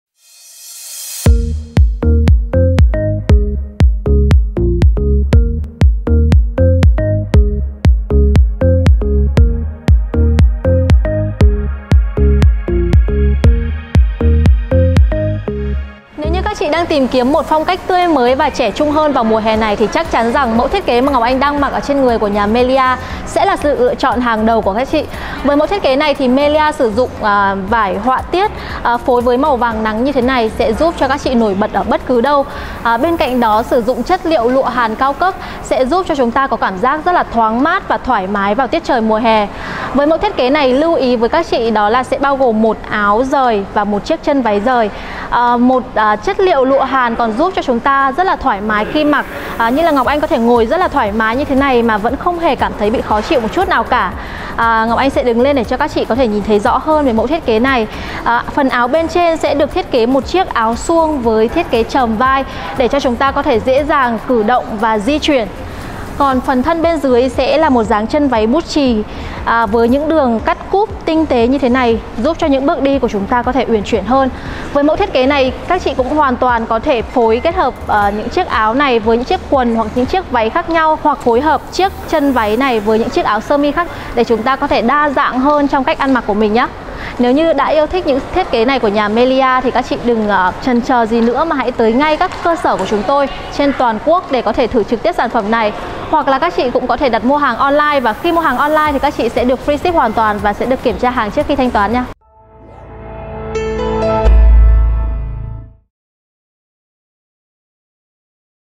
MELYA.VN V210510 - Sắc màu rực rỡ mùa hạ
Chiếc áo hoa cùng họa tiết và chất liệu có thể kết hợp với chân váy, quần trắng tạo lên những set đồ vô cùng mới mẻ ứng dụng nhiều hoàn cảnh khác nhau như đi làm, đi tiệc, đi chơi,...
Chân váy hoa với họa tiết độc đáo và vô cùng sang trọng với sự kết hợp giữ những tông màu mát mẻ cho mùa hè như vàng, màu xanh, màu trắng, phần xếp tà giúp chiếc váy trở lên nhẹ nhàng mỏng manh hơn, chất liệu lụa hàn mỏng nhẹ thoáng mát giúp người mặc cảm thấy dễ chịu cho mùa hè.